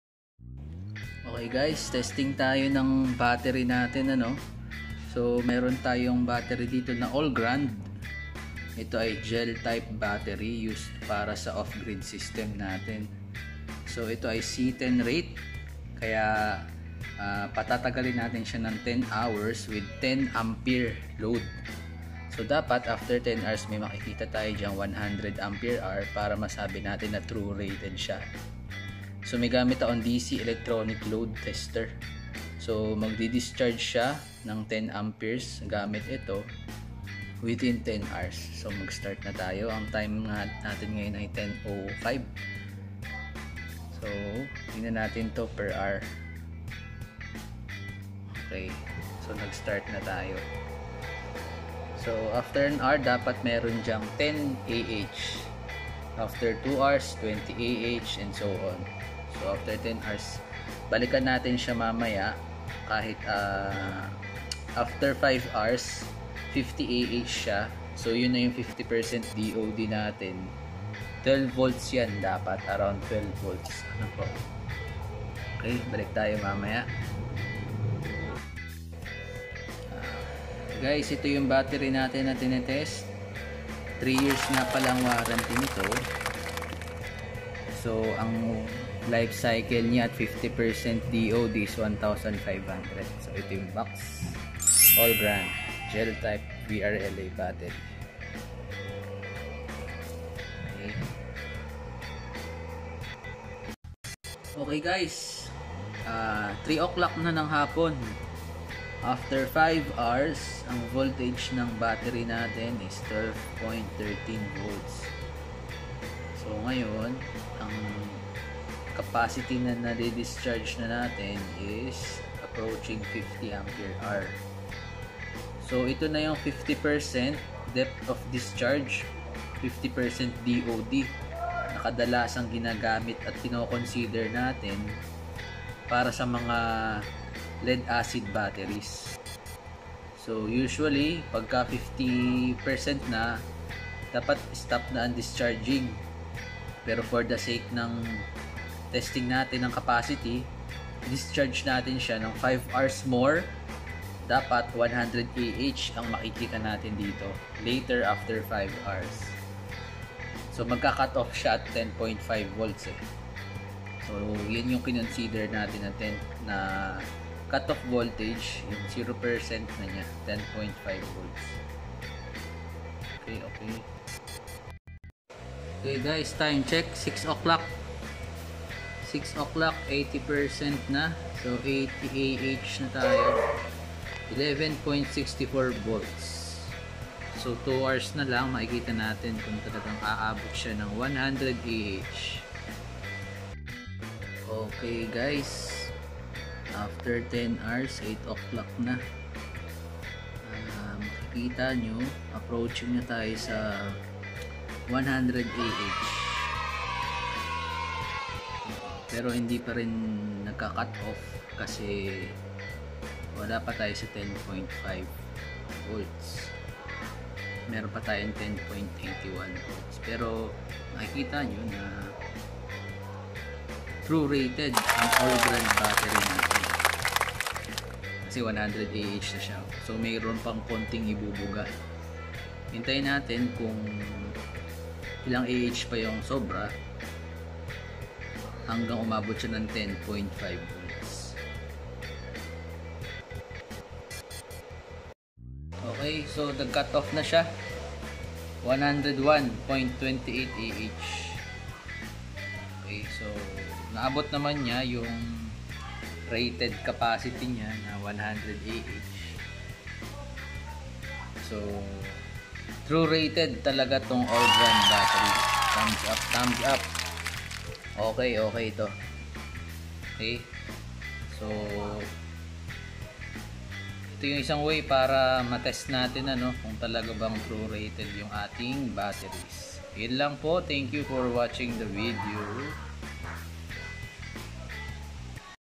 ALLGRAND CAPACITY TEST
Capacity Test of ALLGRAND GEL- VRLA BATTERY using DC Electronic Load Tester with 10 ampere CC (Constant Current) Load.

Start Time @ 10AM
After 5 hours - 50AH with 12.13V
After 10 hours - 100AH with 10.8V

Allgrand Batteries have 3 years warranty.
It has 1,500 cycles at 50% DOD.

Available ang batteries at LEAP ELECTRIC SOLAR SERVICES!

Pls. Check our Facebook Page!

For SOLAR setup you can like our page and join our groups for updates!

Leap-Electric Solar Services
Lucena City